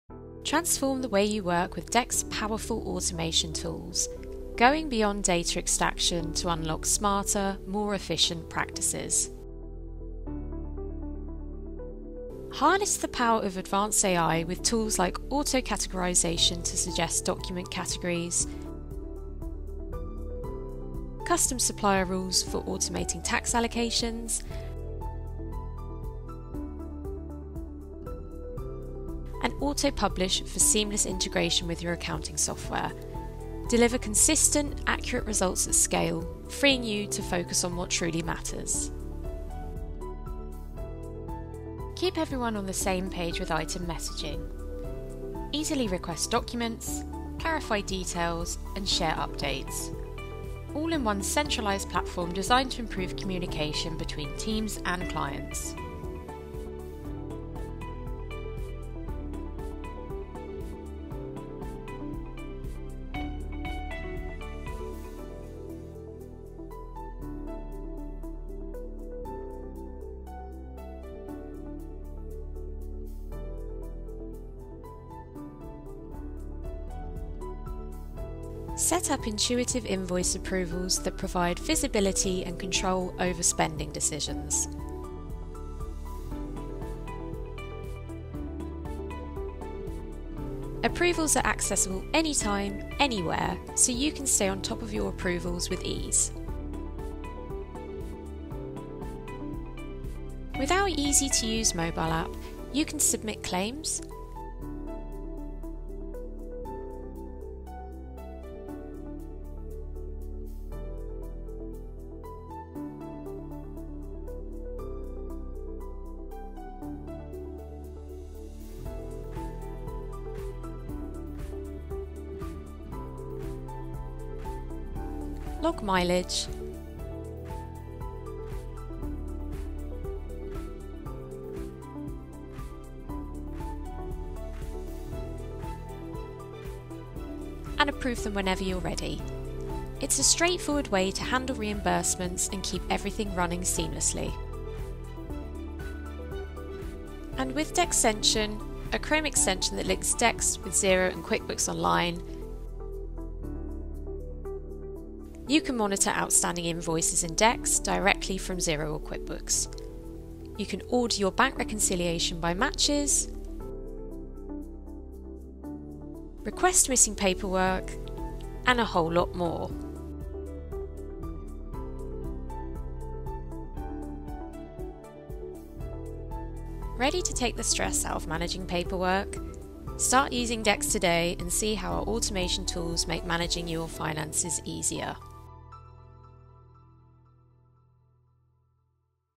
The Evolution of Dext: Bookkeeping Data automation
Say hello to Dext—your all‑in‑one AI-powered bookkeeping automation software. From capture to publish, Dext transforms the way businesses, bookkeepers, and accountants manage financial documents, allowing you to reclaim hours and elevate accuracy.

In this video, you’ll discover:
- How Dext captures invoices, receipts, and bank statements using mobile shot, email, upload, or auto-fetch from retailers, marketplaces, and more.
- How advanced AI extracts and categorises data with 99%+ accuracy, paving the way for seamless workflow.
- How to publish clean, structured data directly to your preferred accounting software—securely stored for up to 7 years.
Why automation replaces chaos—streamlining processes, ensuring accuracy, and freeing you to focus on what matters.

Why it matters:
Eliminates manual entry and paperwork
Delivers fast, precise data with advanced AI
Keeps your records clean, secure, and audit-ready

Who should watch:
Accountants, bookkeepers, and small business owners looking to streamline workflows, reduce admin, and maximise efficiency.